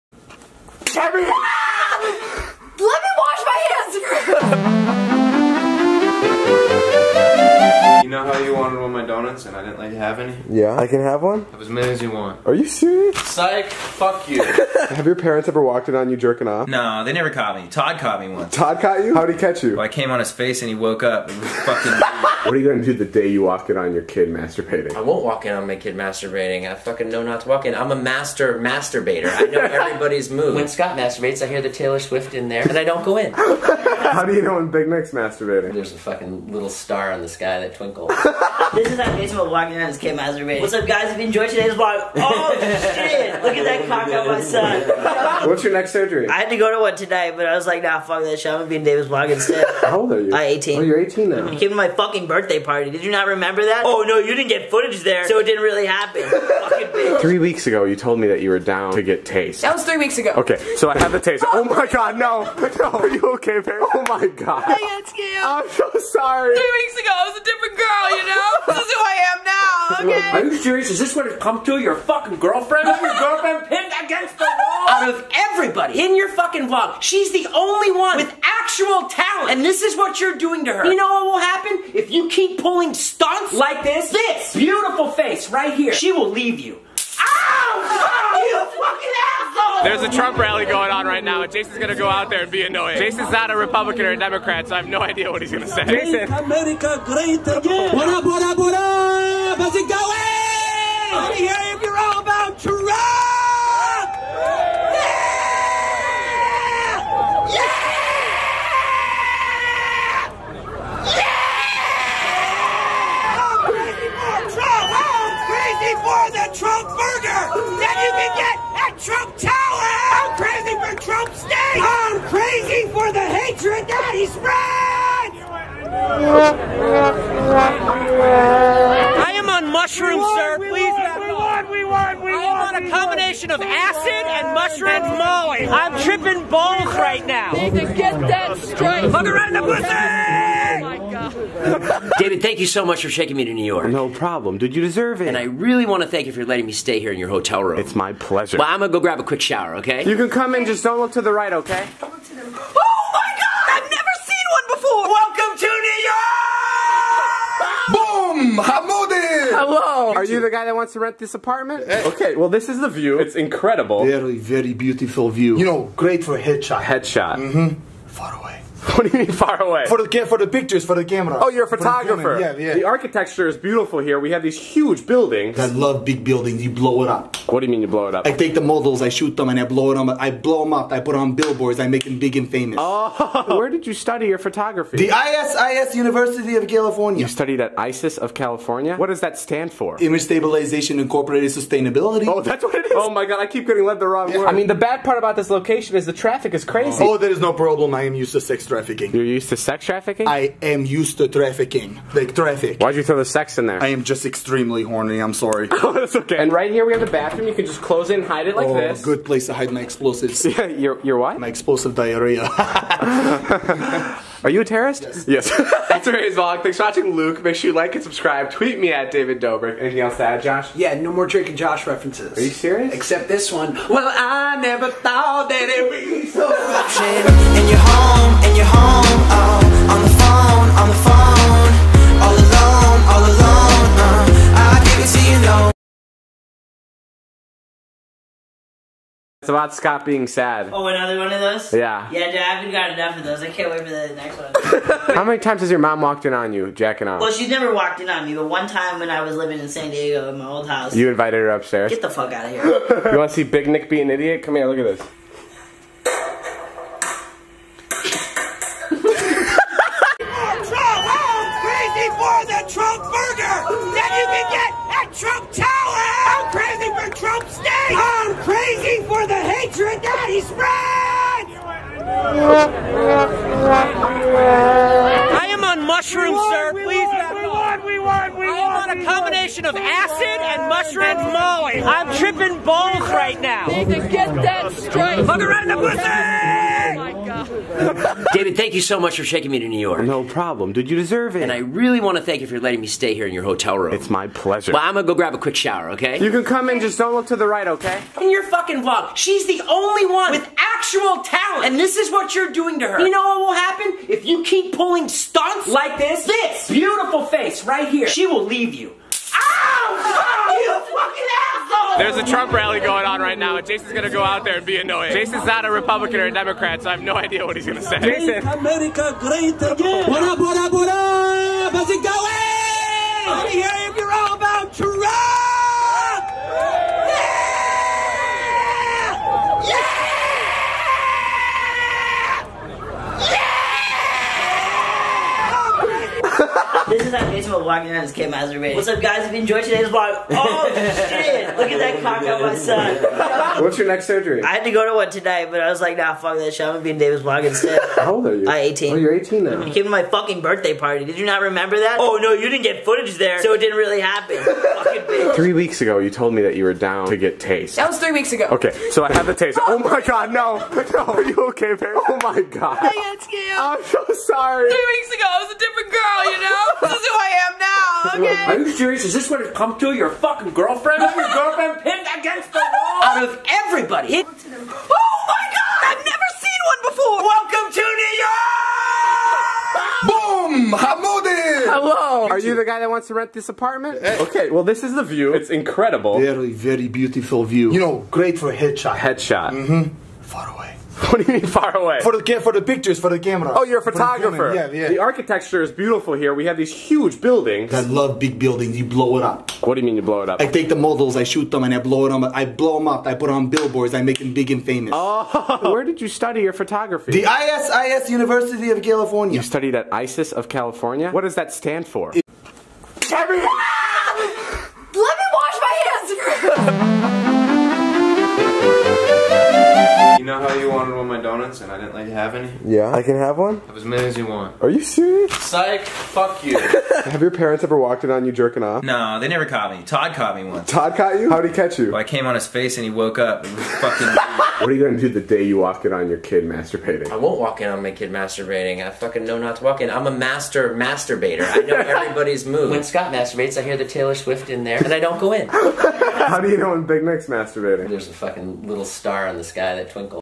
THIS MADE HER START CRYING!!
I mad Liza cry once again.. Jason also interrupts a protest I try selling an apartment.. Alex finally gives me some food.